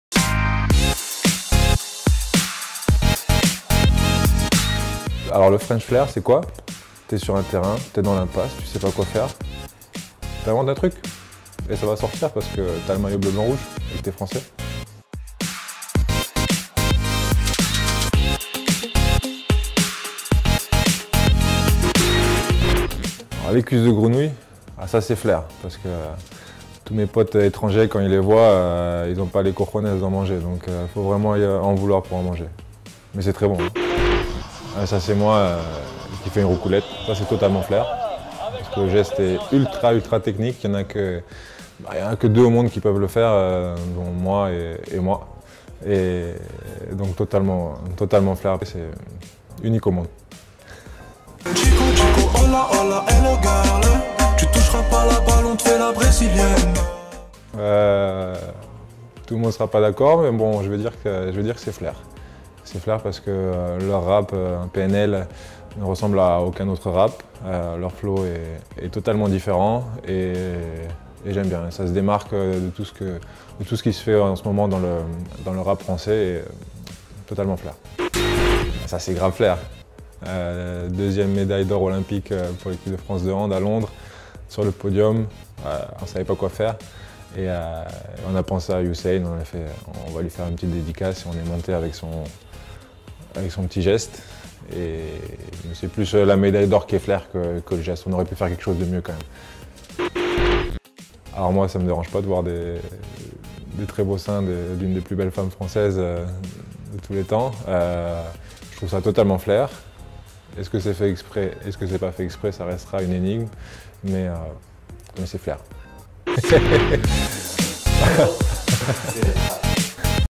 Nikola Karabatic nous partage sa vision du French Flair.
[La Minute French Flair] Nikola Karabatic nous partage sa vision du French Flair. @Bros.agency